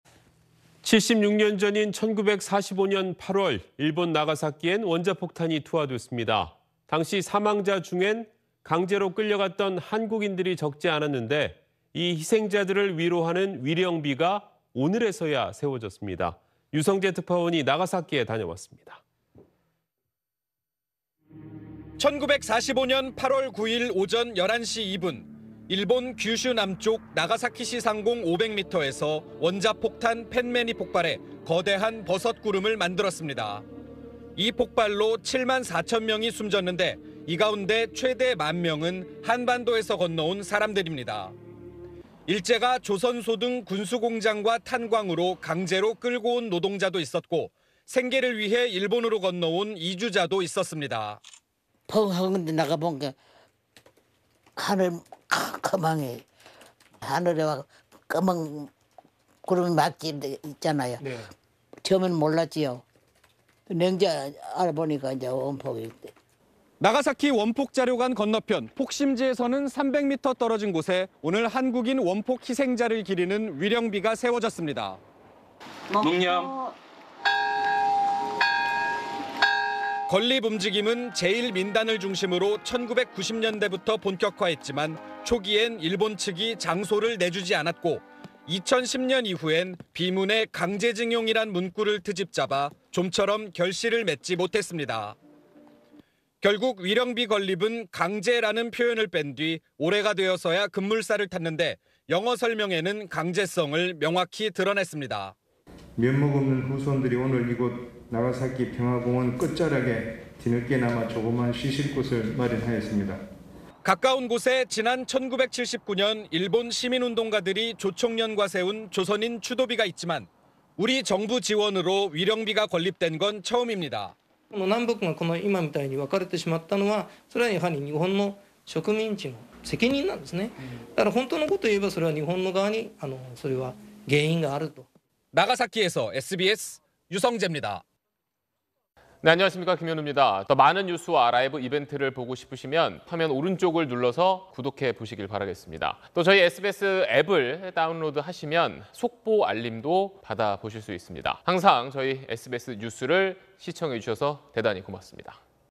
'강제' 표현 빼고…나가사키에 세워진 한국인 위령비 / SBS
76년 전인 1945년 8월 일본 나가사키에는 원자폭탄이 투하됐습니다. 당시 사망자 중에는 강제로 끌려갔던 한국인들이 적지 않았는데, 이 희생자들을 위로하는 위령비가 오늘(6일)에서야 세워졌습니다.
유성재 특파원이 나가사키에 다녀왔습니다.